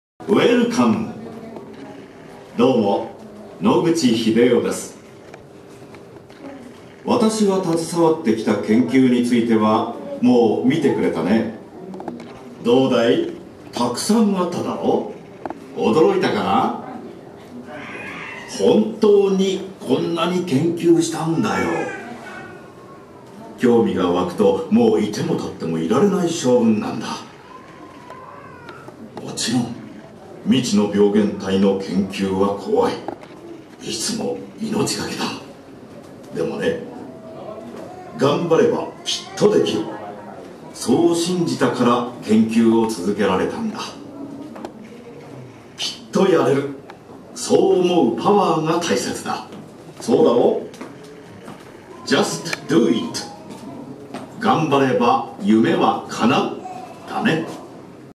野口英世の記念館にてロボット
野口秀雄記念品館にて、ハイテクを駆使した感じのロボットです。
なかなか面白い感じですね。